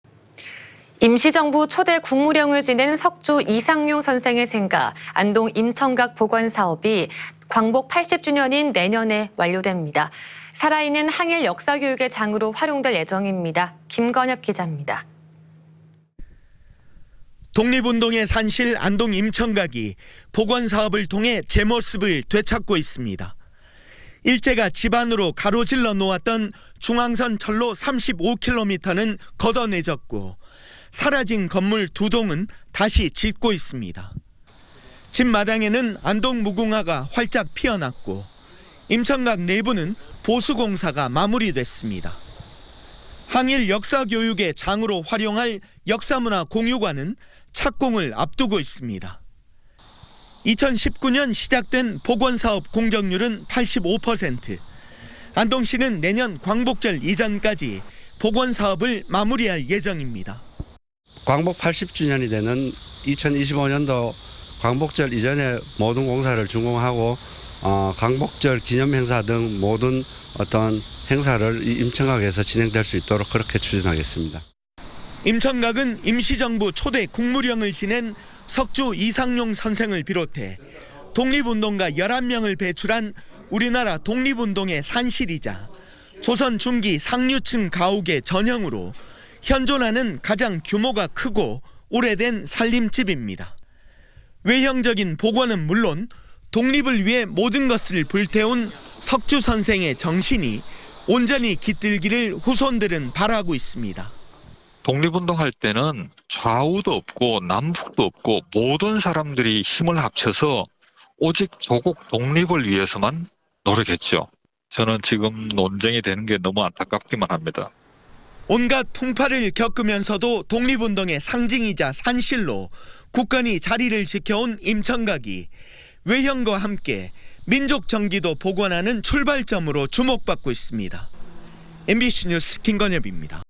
독립운동 산실 안동 '임청각' 내년까지 복원 완료/ 안동MBC
2024/08/14 12:00:00    작성자 : 김건엽

독립운동의 산실인 안동 '임청각' 복원사업이
광복 80주년을 맞는 내년까지 완료됩니다.

현재 공정률은 88%로
임청각 좌우측에 재현가옥 2동을 복원하고
일제의 철도개설로 훼손된 주변 지형과
수목 재정비하는 작업이 진행 중입니다.

또 석주 선생의 독립정신을 기리고
교육공간으로 활용할 역사문화관도
건립되고 있습니다.

안동시는 광복 80주년인 내년까지
복원사업을 마무리하고 역사교육의 장으로
활용할 계획입니다..